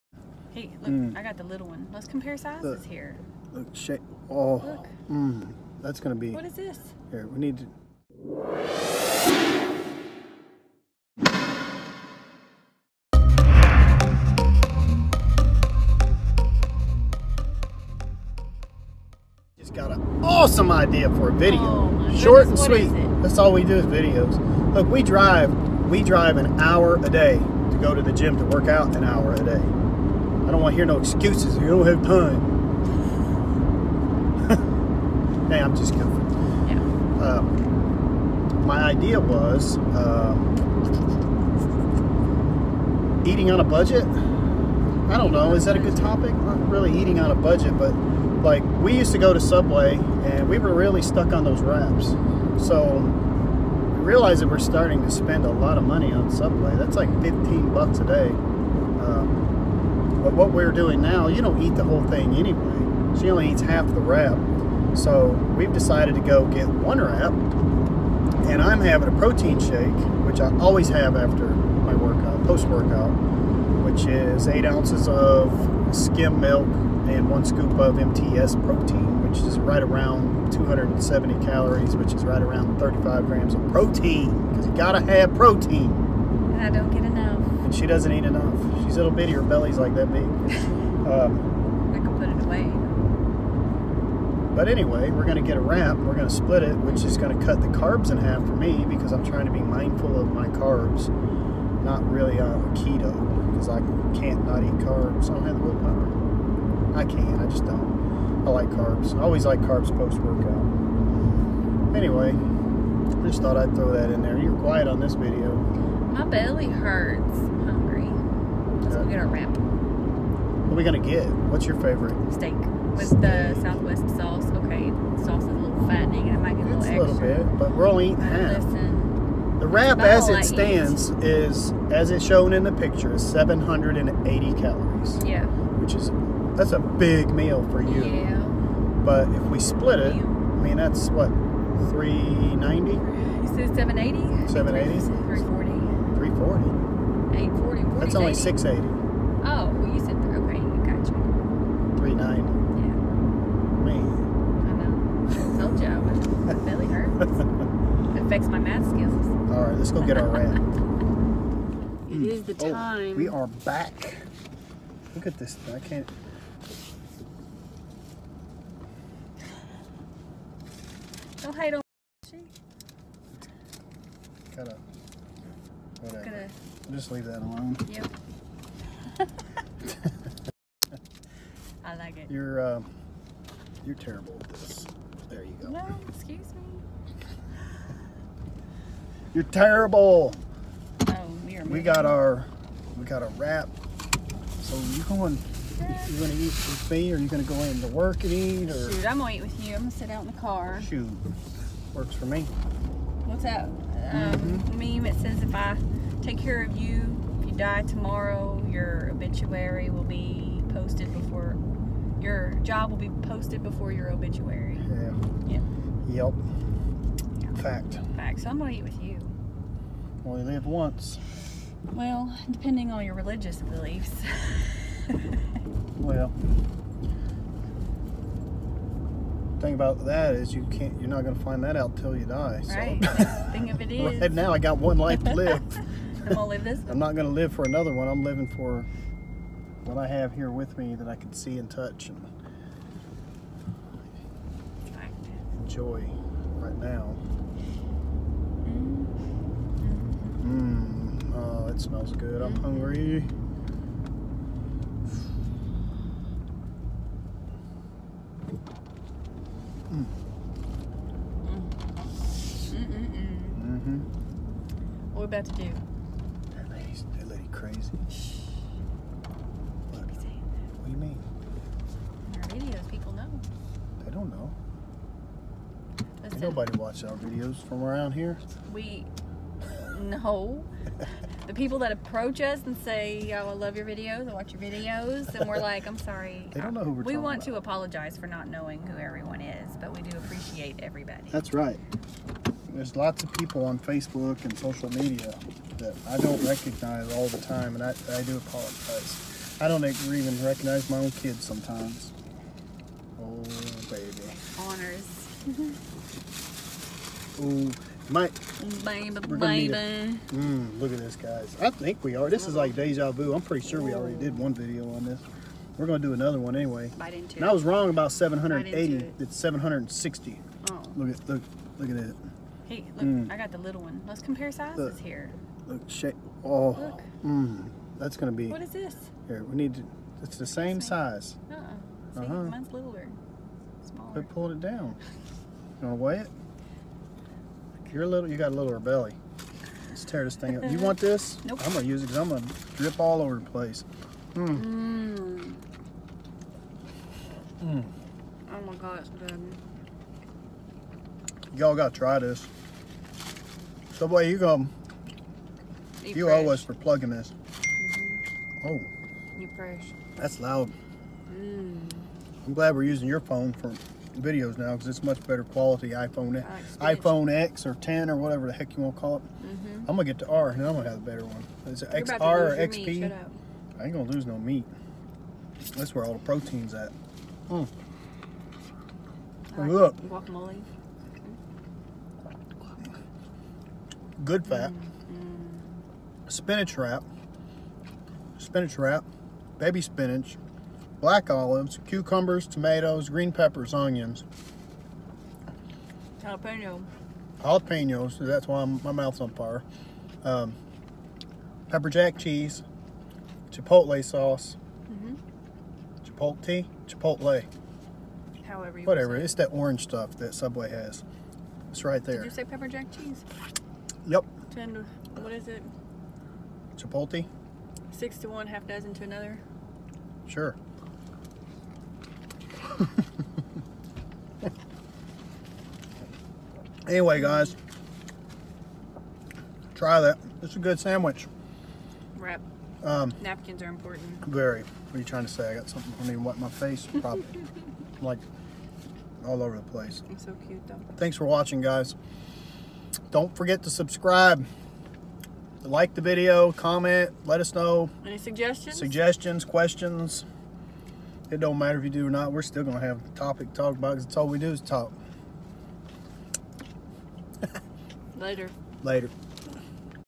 Eating on a budget | Subway
There’s never enough time so fast food it is and let’s be mindful of cost! If you’ve forgotten to meal prep or just have a busy life like the rest of us, check out how it’s possible to eat moderately healthy on the go.